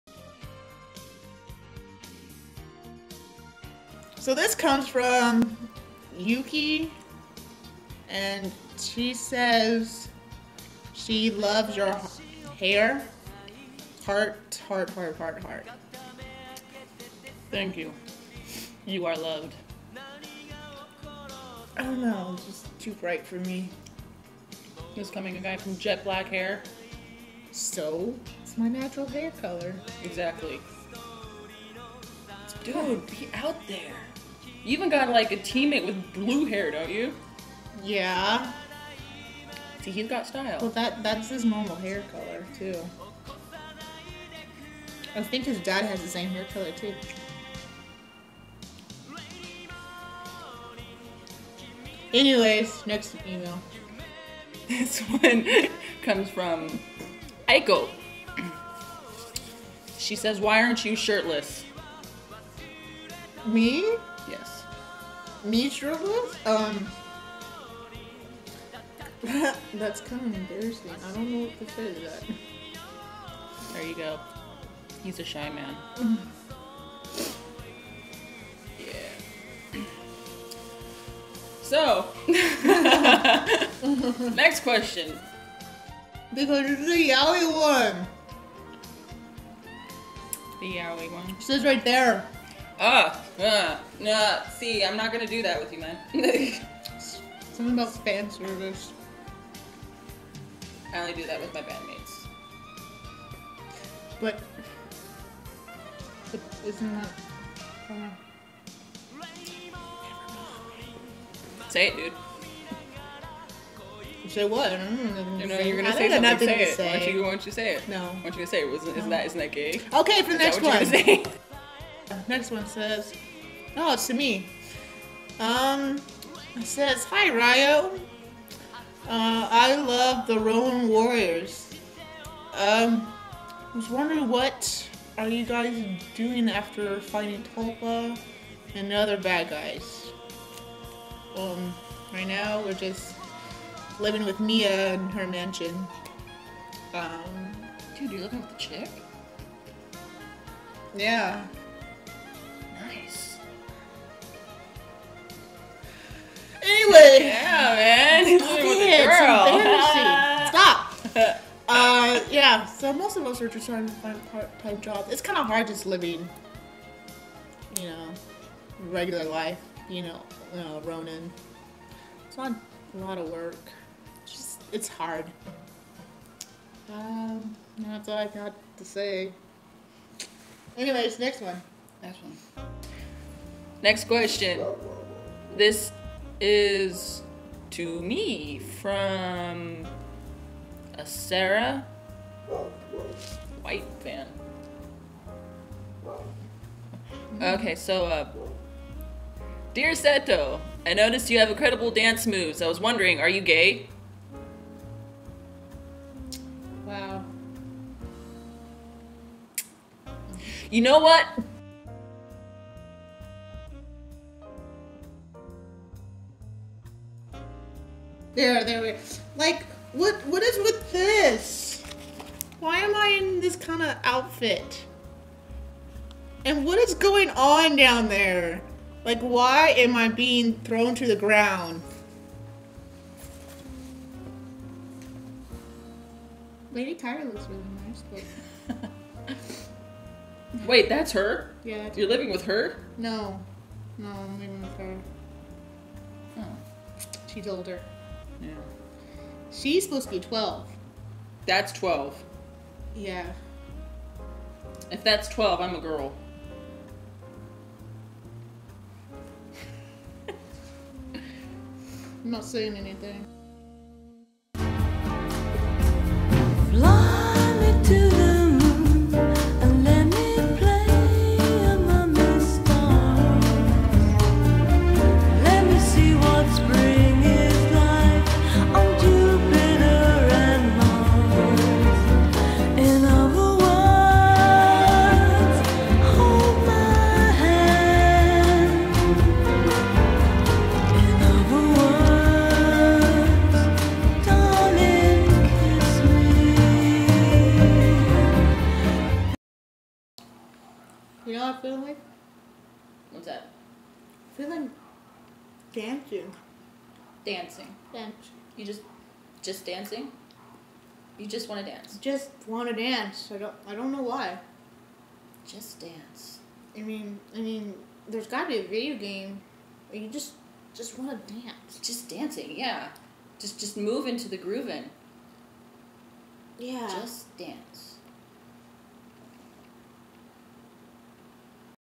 Valentines Day with Seto and Ryo two
Part 1! Hello everyone! Knight here, wishing you all a happy Valentine's Day! Here is a special time with your smexy boys of the hour, Jpop idol Seto and fiery Ronin leader, Ryo Sanada!